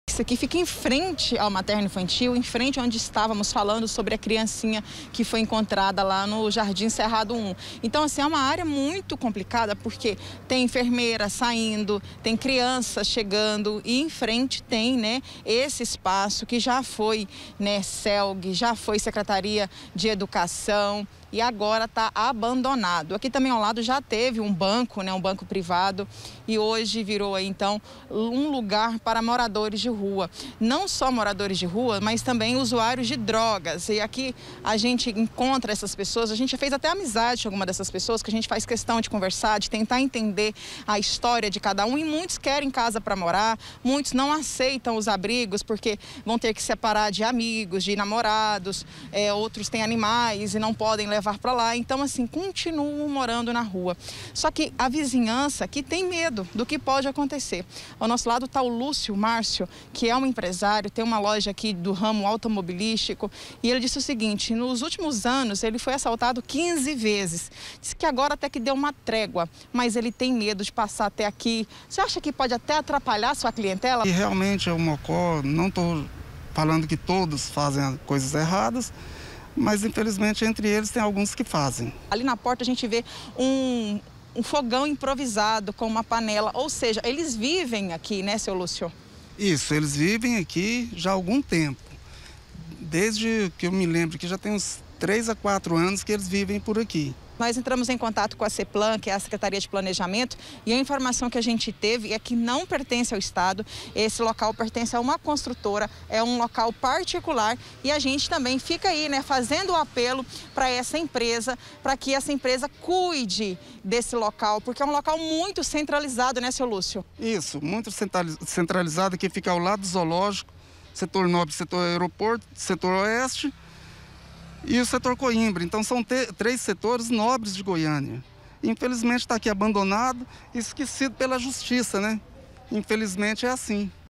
VIZINHO INDESEJADO - PRÉDIO ABANDONADO VIRA MOCÓ E PROVOCA PROBLEMAS.
PRÉDIO ABANDONADO NO SETOR COIMBRA TIRA O SOSSEGO DE MORADORES E COMERCIANTES. O LOCAL ONDE JÁ FUNCIONOU A SECRETARIA ESTADUAL DE EDUCAÇÃO VIROU MOCÓ E PROVOCA MUITOS PROBLEMAS.